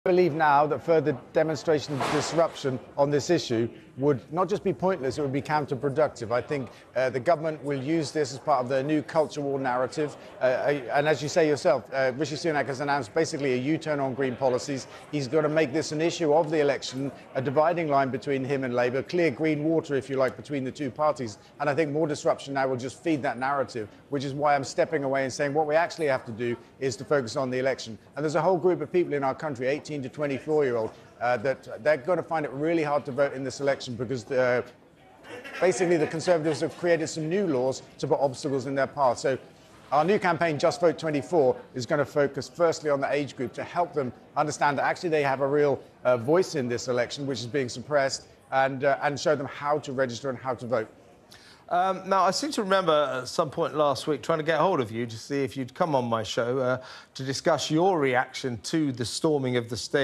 Just Stop Oil ‘Pointless’ And ‘Counterproductive’ Says Dale Vince After Stopping Donations To Group
Just Stop Oil ‘Pointless’ And ‘Counterproductive’ Says Dale Vince After Stopping Donations To Group On Talk TV.